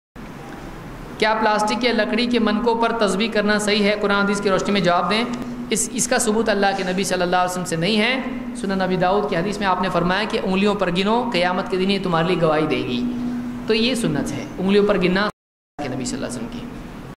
Kya plastic ya lakdi ke manko par tasbeeh karna saheeh hai | Abu Zaid Zameer
This Questions and Answer is taken from :
Lecture : Haram cheezen jinhen halka samajh liya gaya hai
Date :  Thu, 31 Jan 2013.
Place : Bijapur.